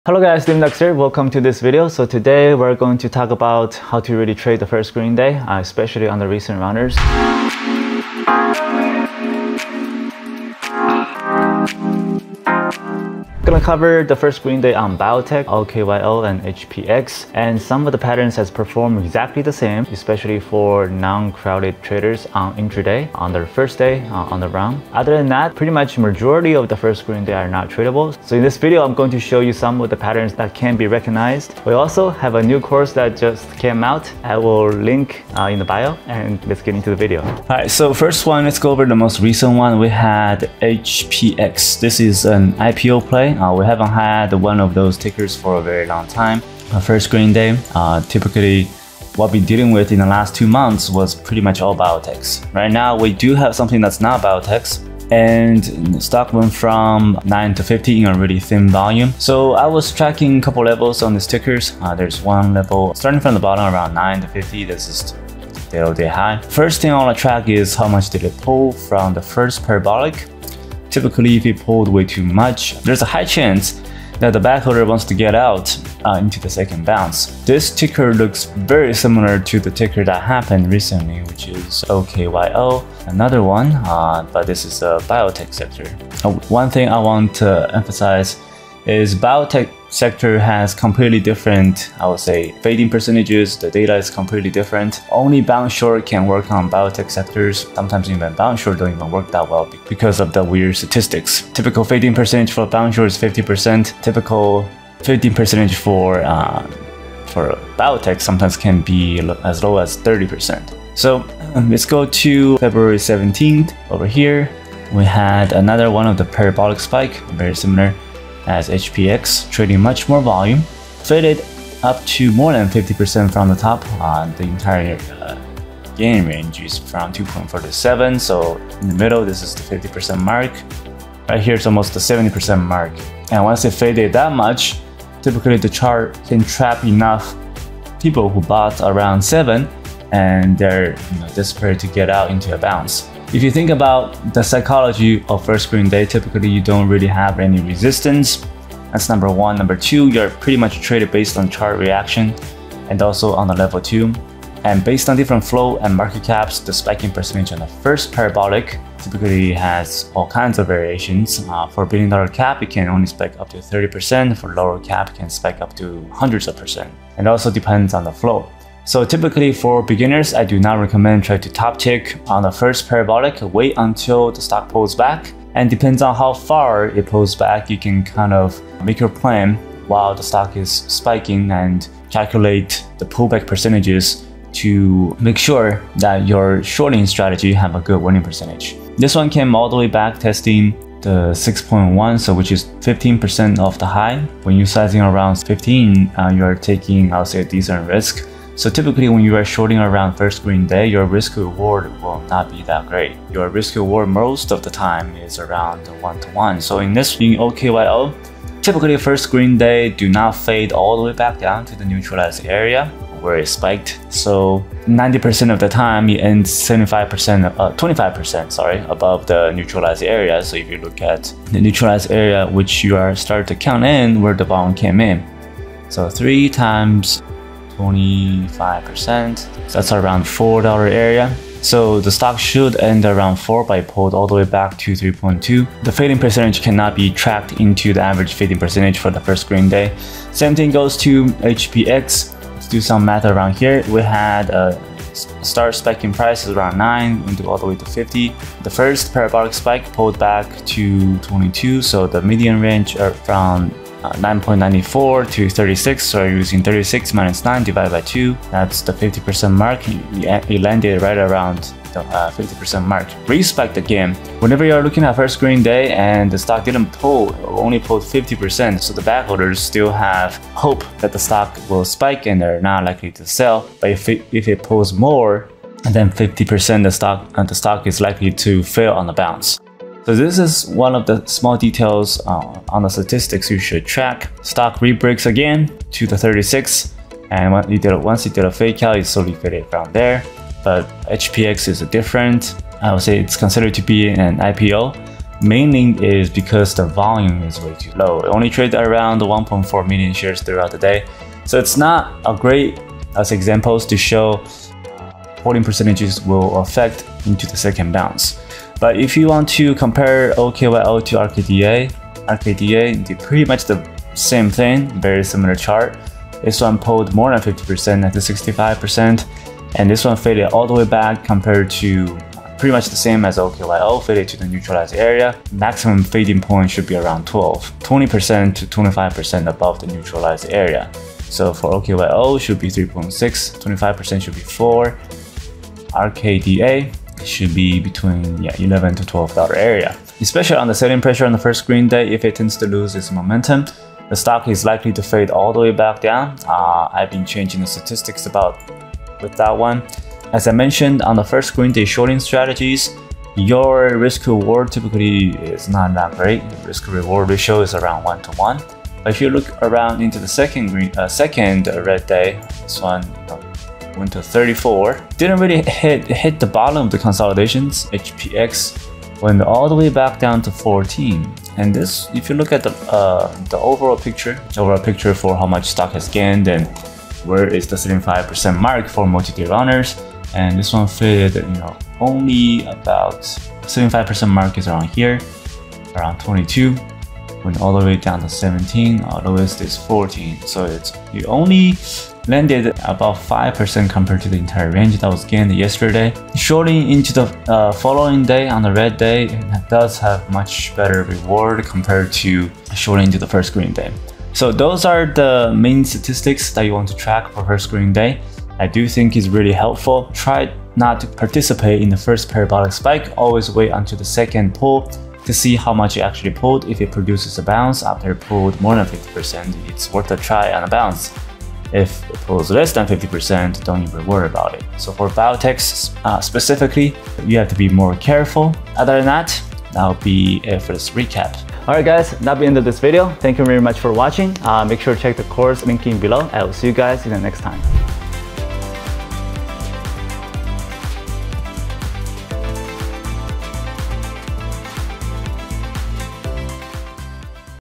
A Profitable Trading Strategy Revealed: The First Green Day!
🎥 In this engaging video, we reveal the secrets 🤫 to mastering the first green day phenomenon and guide you in crafting a profitable trading strategy 📈 tailored to this unique market occurrence.

In our all-inclusive guide, we'll explore:

1️⃣ Understanding the First Green Day: What it is, why it's important, and how it can influence your trading decisions 💡
2️⃣ Analyzing market trends 📊 and pinpointing the perfect first green day setups for maximum gains 💰
3️⃣ Step-by-step guidance 🚶‍♂️ for creating a customized trading plan to keep you disciplined and goal-oriented 🎯
4️⃣ Risk management strategies 🛡️ to safeguard your portfolio and optimize your profits
5️⃣ Real-life examples 🌟 and case studies demonstrating the power of the first green day strategy in action

We do not have any other social media accounts.